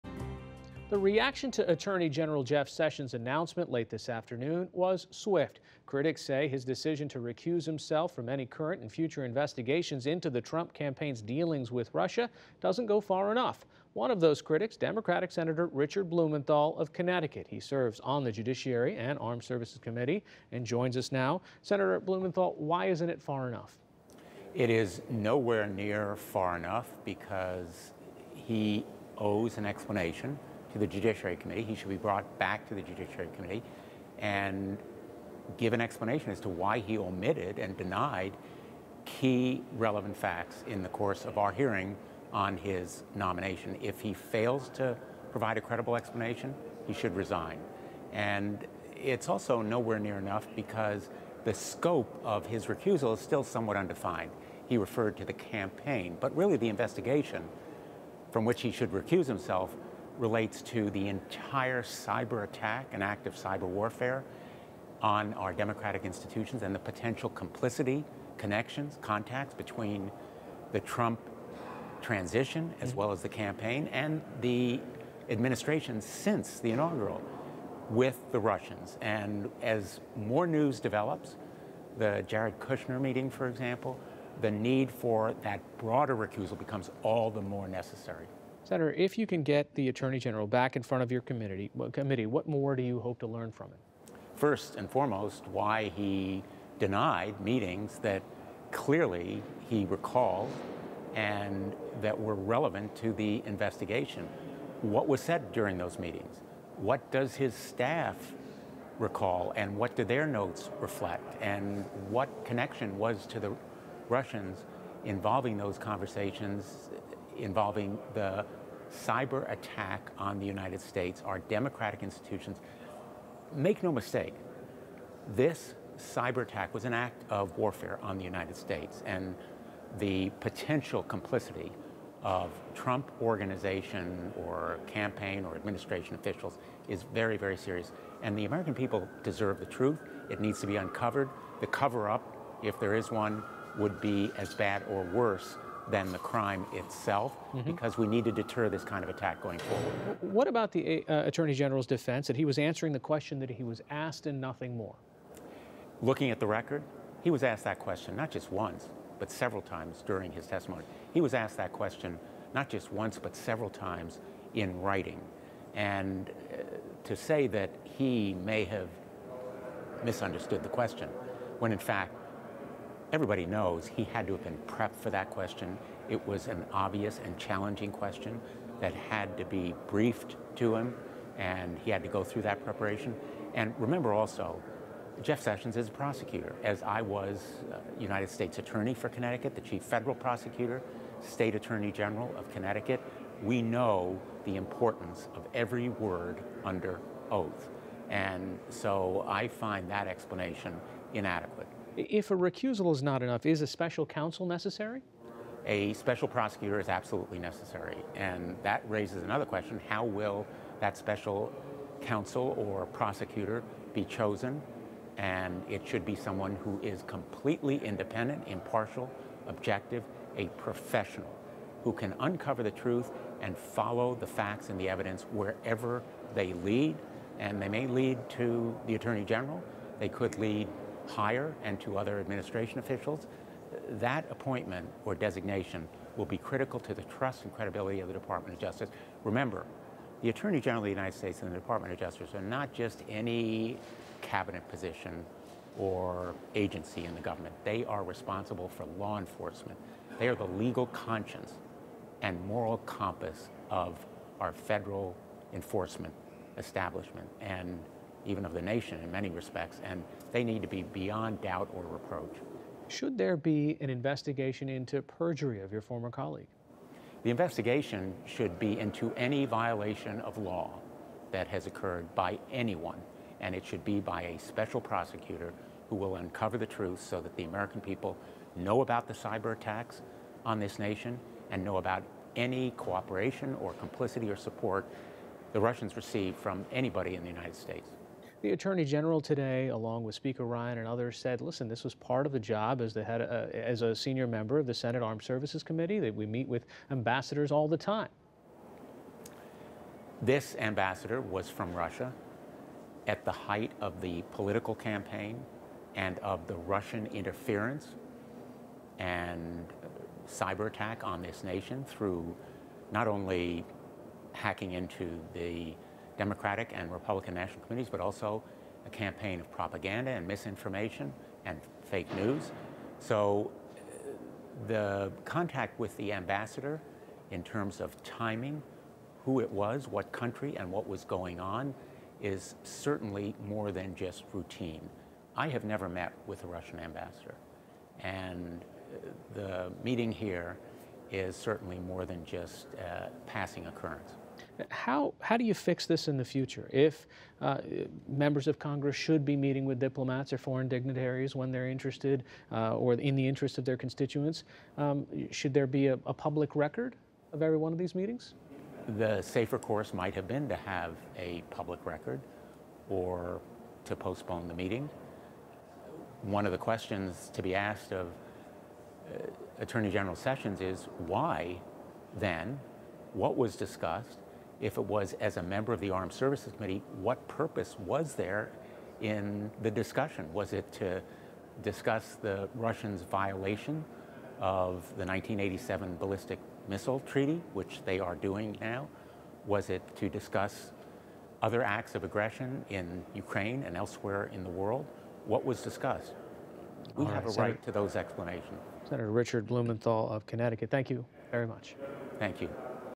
Blumenthal: Sessions needs to give ‘credible explanation’ or resign
Critics say Attorney General Jeff Sessions' announcement to recuse himself from any investigations related to the Trump campaign doesn't go far enough. Hari Sreenivasan speaks with Sen. Richard Blumenthal, D-Ct., one of those critics, about what he thinks Sessions needs to do now.